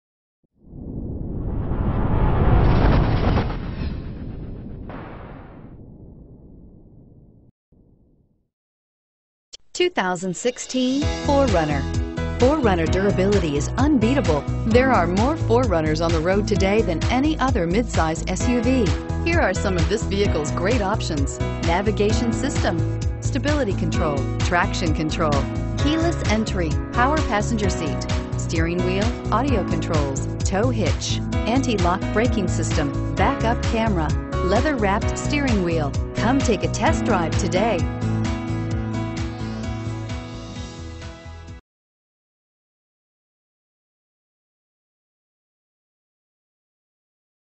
2016 Toyota 4Runner Metairie LA GL1717
2016 Toyota 4Runner SR5 Premium

Lakeside Toyota
3701 North Causeway

Looking for the right sport utility? Today could be your lucky day. With 0 miles, this Super White, 2016 Toyota 4Runner equipped with Automatic transmission could be yours. Request more information and set up a test drive right away.